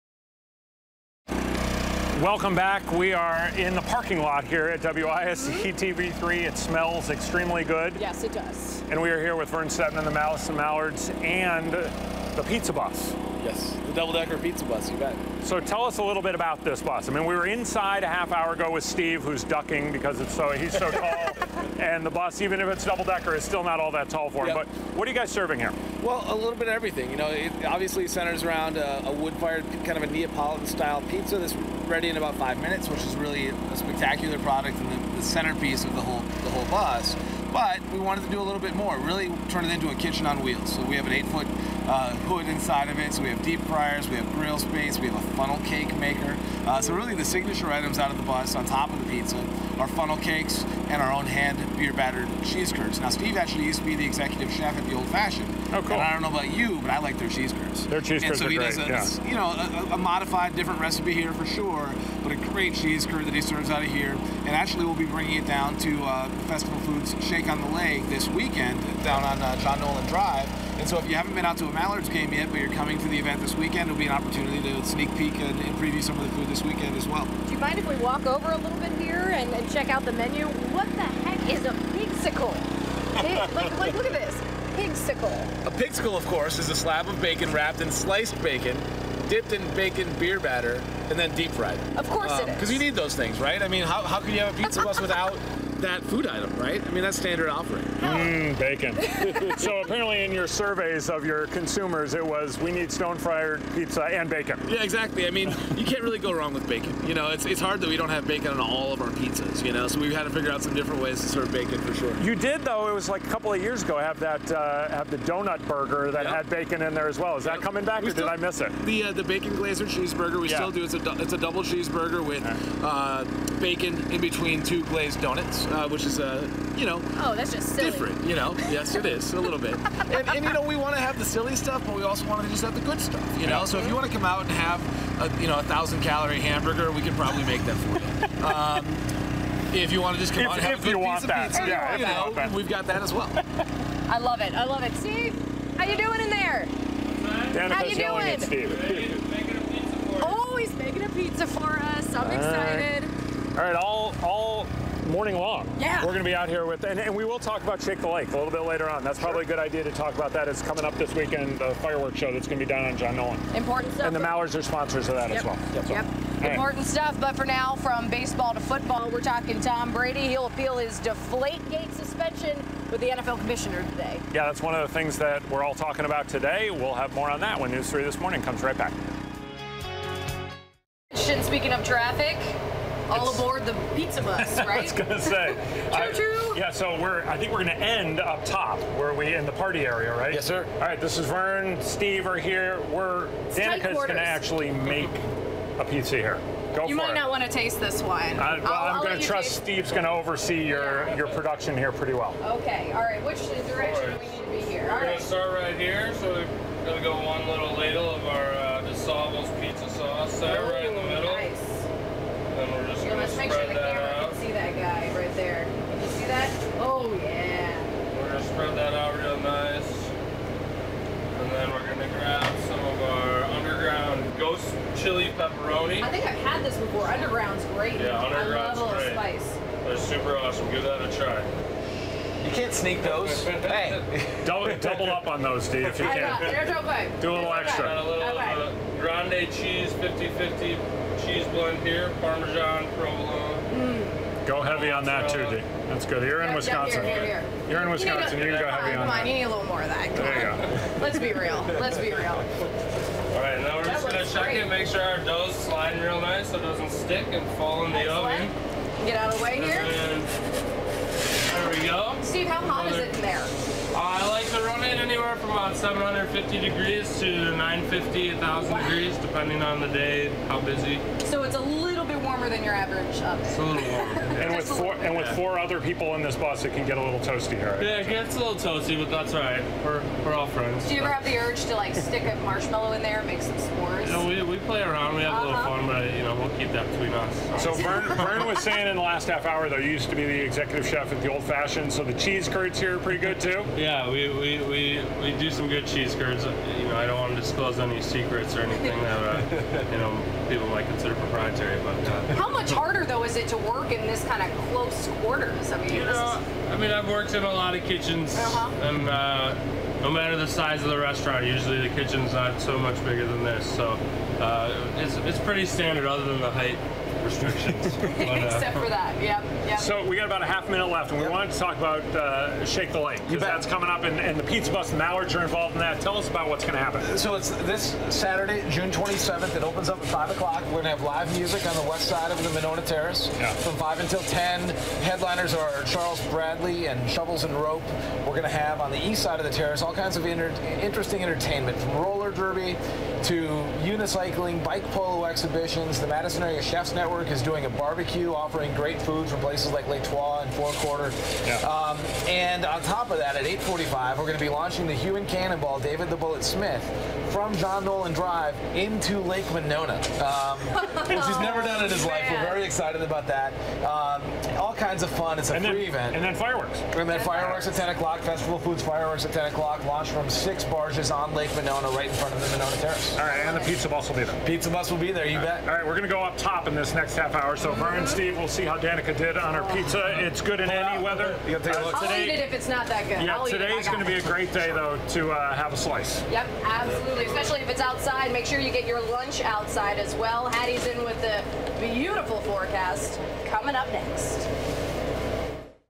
Check out the Mallards' double decker pizza bus
The pizza bus stopped by News 3 studios Tuesday to showcase the double decker pizza bus and their ballpark menu.